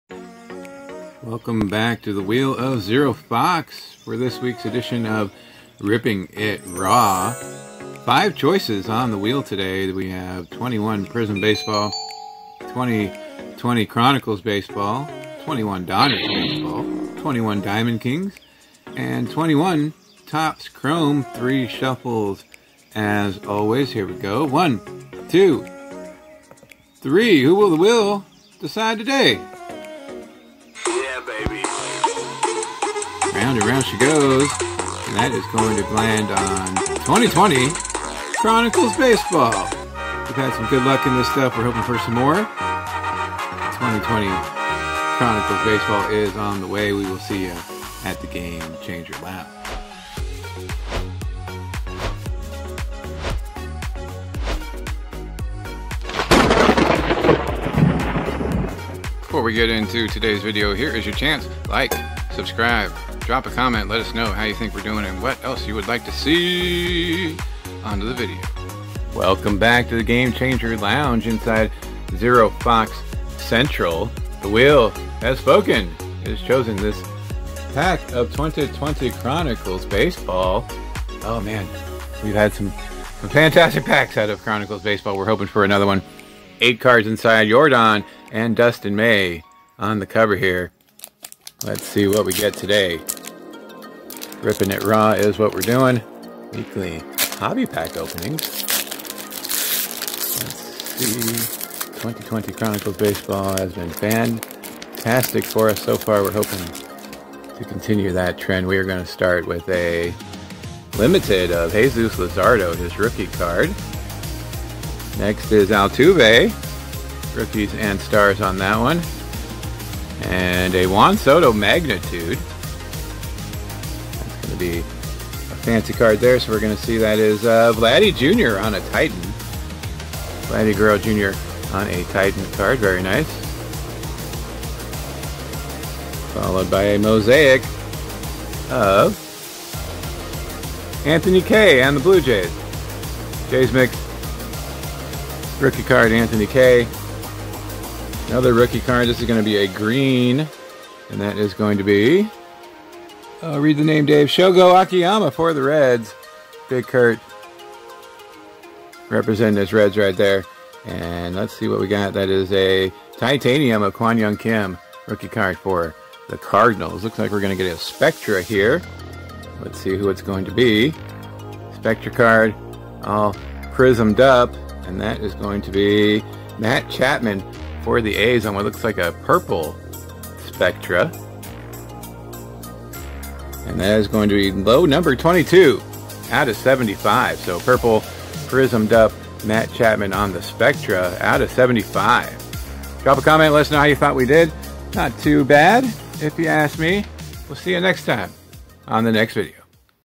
Ripping it Raw ep. 18 /25 & /75 pull
the wheel of zero Fox decides which hobby pack we open on this 18th episode of ripping it Raw